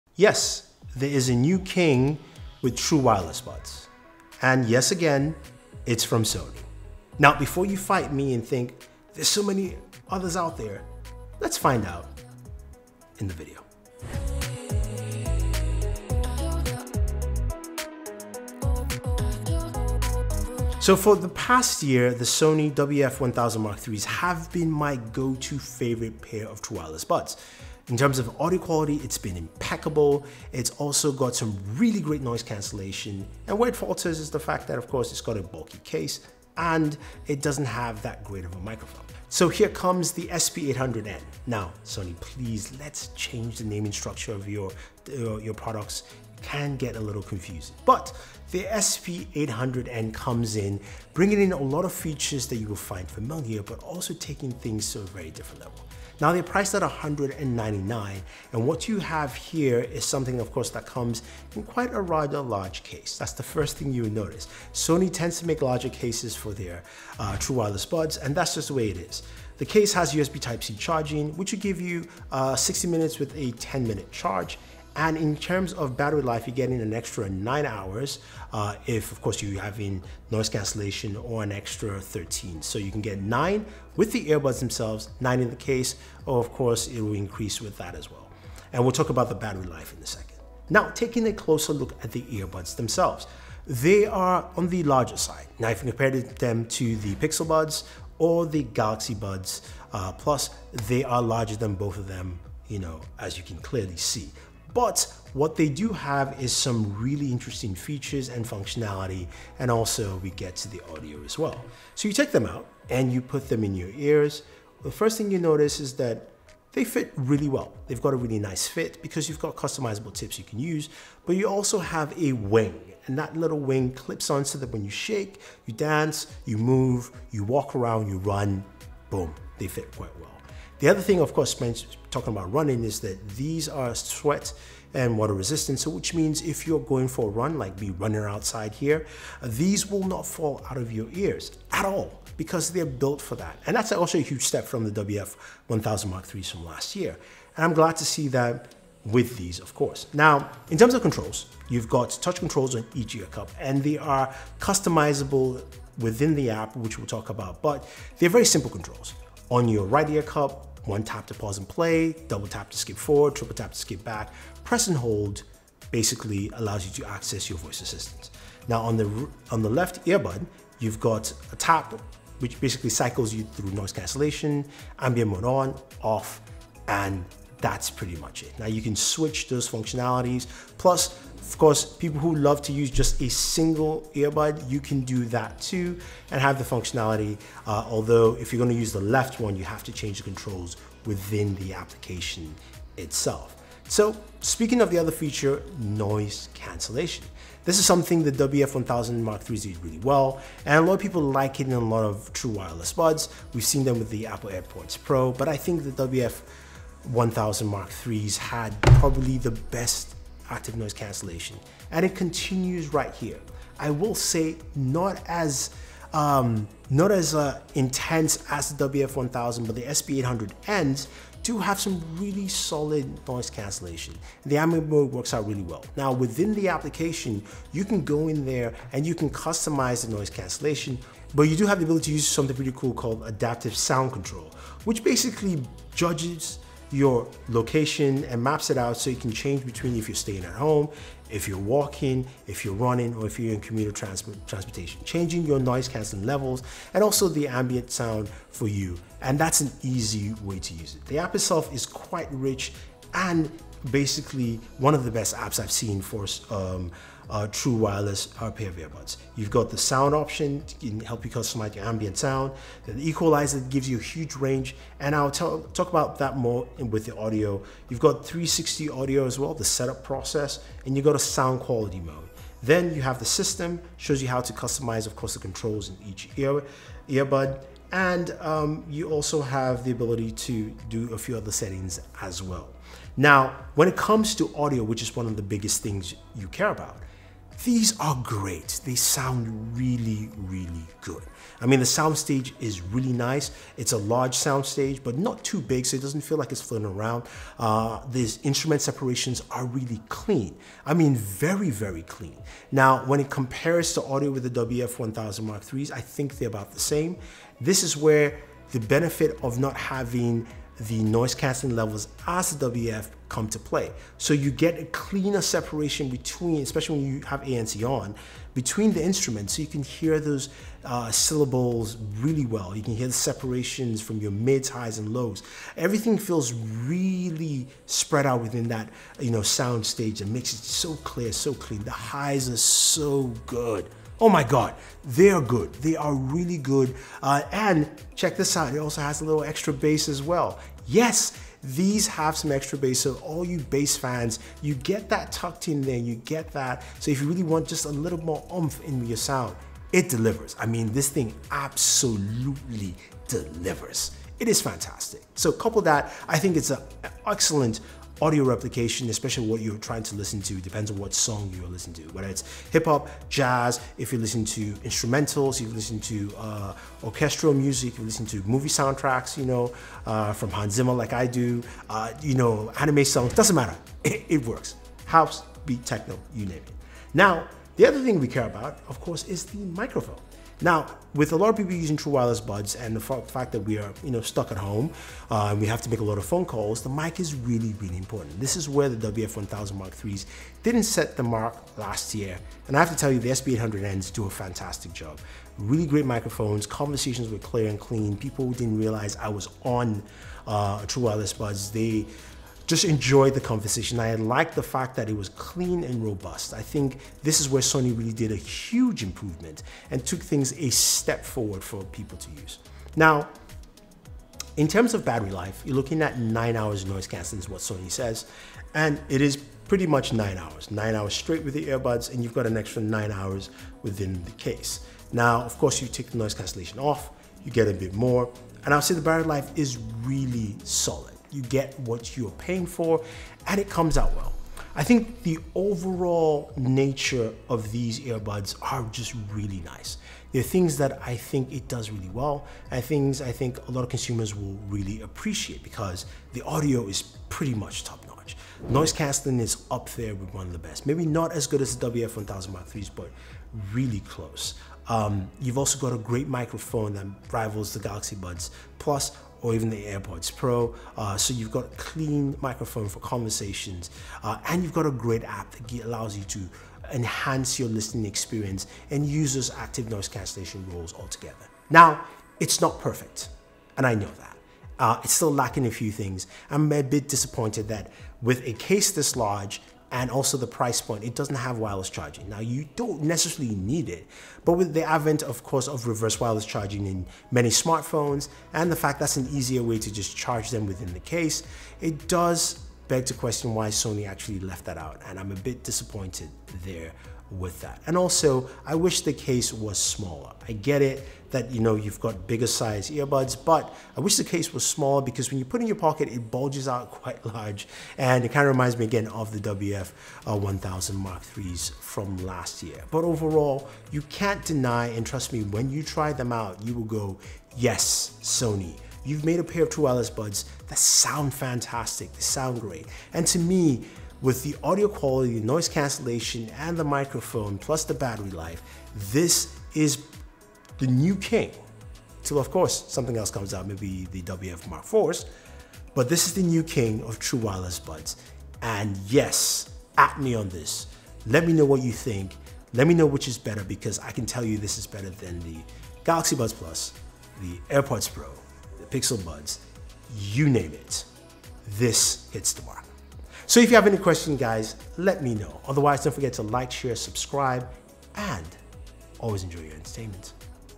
SONY WF-SP800N Review | New True Wireless King???
The Sony WF-SP800N marks an evolution in true wireless audio that sets ti apart from the competition. But does that warranty the claim of being the Best True Wireless Headphones? Let find out.

Sony WF-SP800N
Google Pixel Buds
Galaxy Buds+

1) iMobile Test drive: A day with Mate 20 Pro +A Surprise